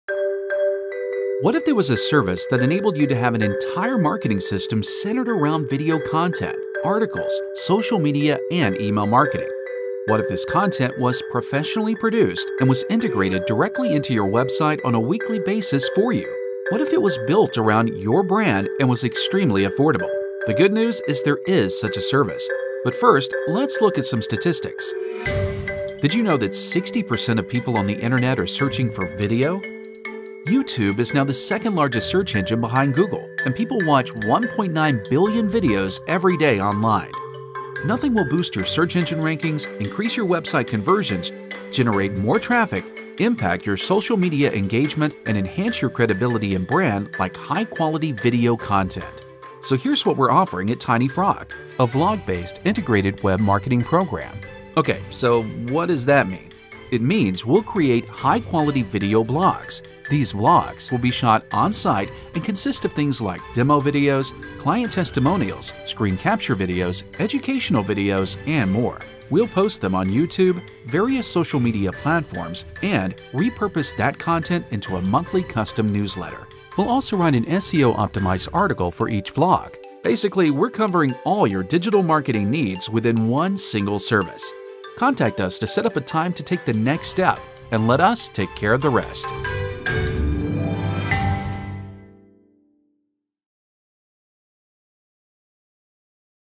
1 TinyFrog VlogMarketing H264
What if there was a service that enabled you to have an entire marketing system centered around video content, articles,
social media, and email marketing? What if this content was professionally produced and was integrated directly into your
website on a weekly basis for you? What if it was built around your brand and was extremely affordable?

The good news is there is such a service. But first, let's look at some statistics. Did you know that sixty percent of people on the
internet are searching for video? YouTube is now the second largest search engine behind Google and people watch 1.9
Nothing will boost your search engine rankings, increase your website conversions, generate more traffic, impact your social media engagement, and enhance your credibility and brand like high-quality video content.

So here's what we're offering at Tiny Frog: a vlog-based, integrated web marketing program. Okay, so what does that mean? It means we'll create high-quality video blogs. These vlogs will be shot on site and consist of things like demo videos, client testimonials, screen capture videos, educational videos, and more. We'll post
them on YouTube, various social media platforms, and repurpose that content into a monthly custom newsletter. We'll also write an SEO optimized article for each vlog. Basically, we're covering all